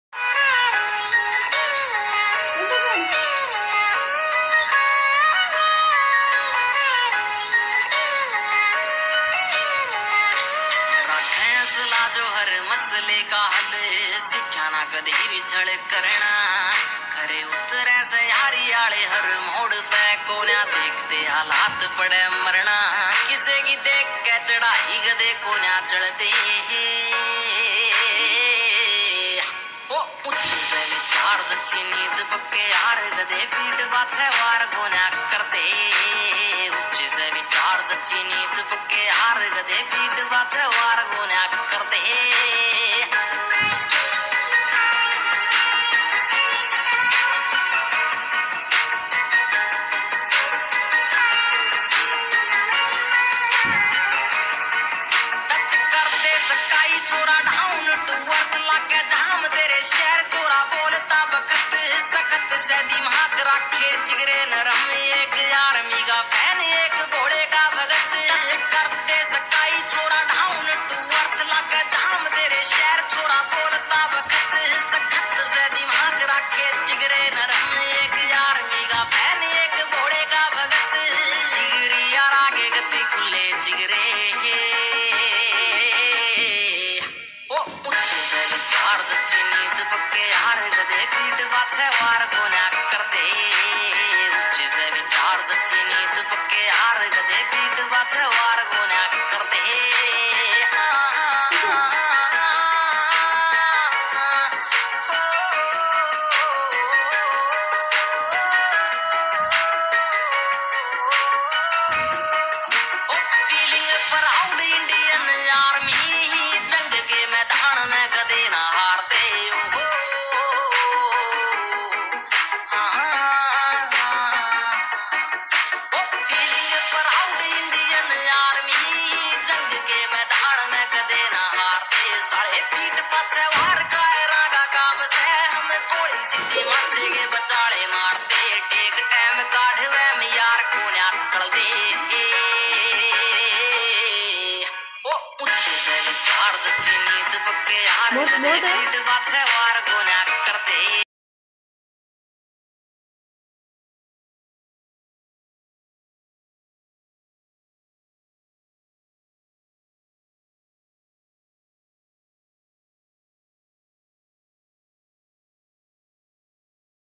DancingDelight - Abdul Raafe Bagwan (Adorables)
Encouragement Eduserve

Pride is having children picking up skills that makes one go wow.
Abdul Raafe Bagwan, a student of Indian Public School Shahpura, is here to amaze us with his wonderful dance performance.

Congratulations to the School and kudos to the parents and the family. A huge shout out to the family members and the parents who made this happen.
Encouragement Eduserve wishes him all the very best for his future endeavours.

Also Like and Review us on Facebook.